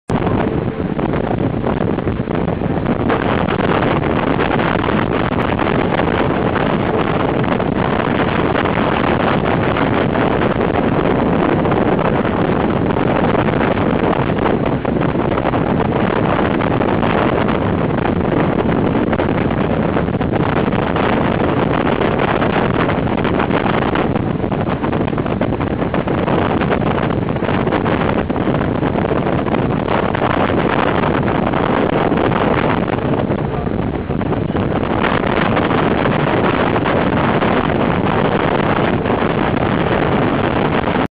Malaysia 2008 - Überfahrt zu Pulau Perhentian
Speedboot fahrt zu Pulau Perhentian Kecil